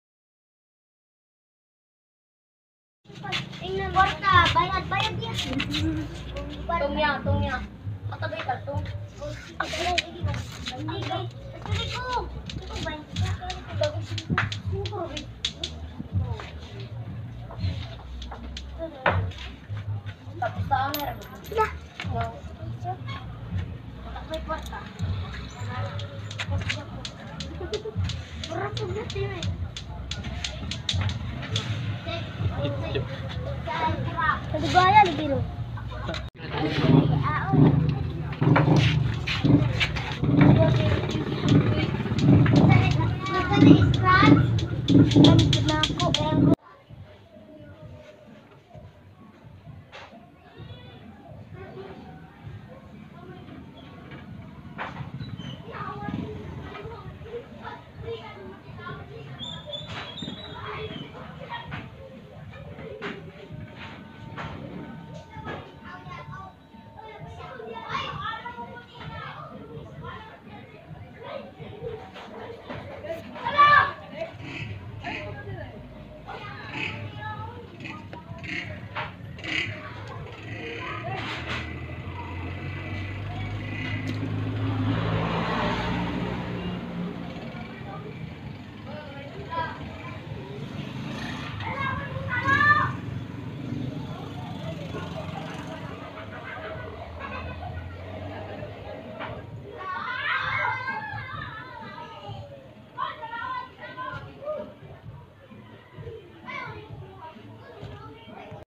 Store update#1:Palabunotan patok sa aking sari sari store#viral
Hello mga ka Negosyo!Palabunutan or Bunot bunot ay patok sa negosyo ngayong tag ulan plus added income sa ating tindahan may tubo ka na malaki and you can play with the kids talagang hindi ka talaga aantukin👍gonegosyo

More videos to watch👇

*Store update update/Checking and restocking
of delivery